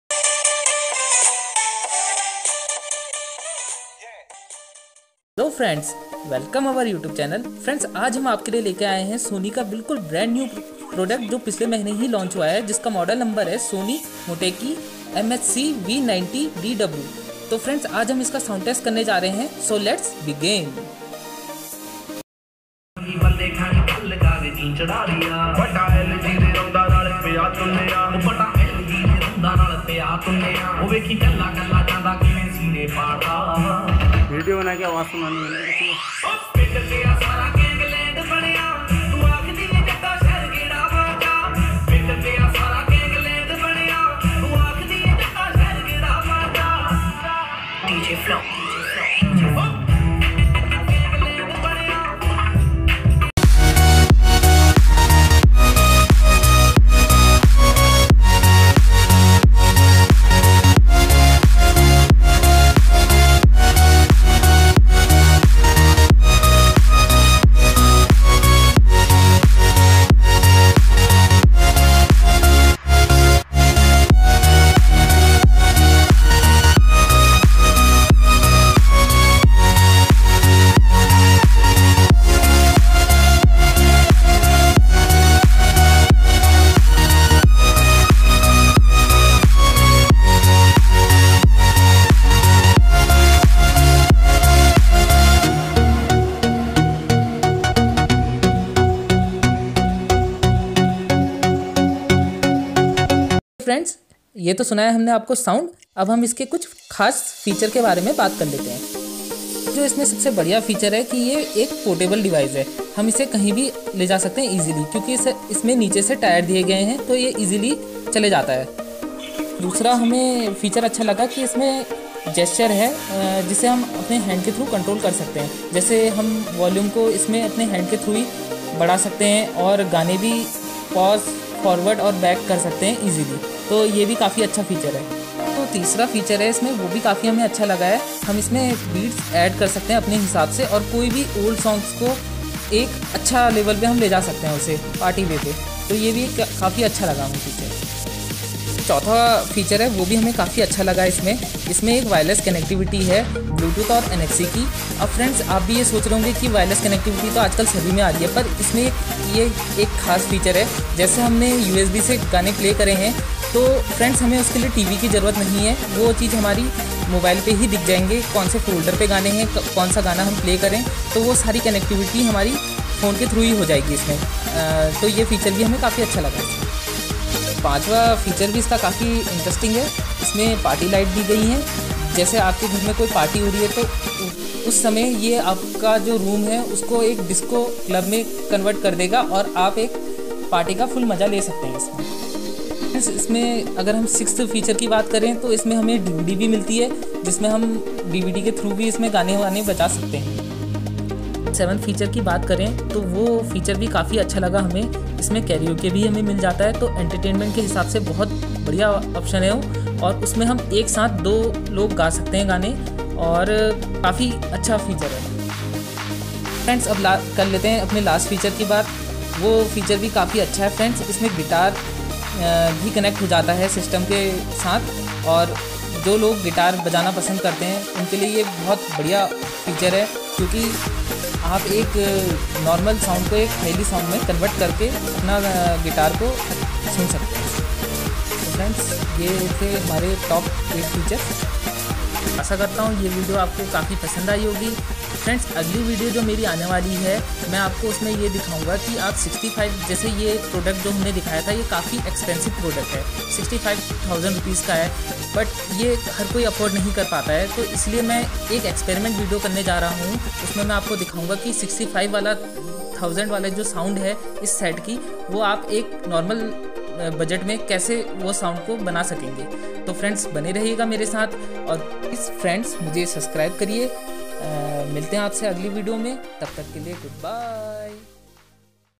Sony muteki MHC-V90DW review, sound test,(TOP 8 FEATURES IN HINDI)
My GEARS-

Selfie stick
Sound test Mic

Frndz aaj hum aapke liye SONY ke brand new music system ka sound test leke aaye hai.Frndz aap is video ko dekh ne k leye aapne headphone ka use kare taki aapko sound ki quality ka pata chal sake.